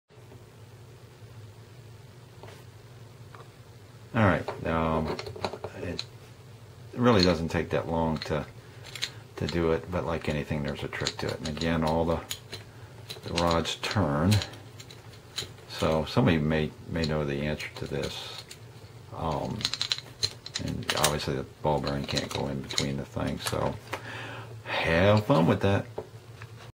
Escape from Alcatraz - Part 2
Puzzle box where you remove a large ball bearing from a box.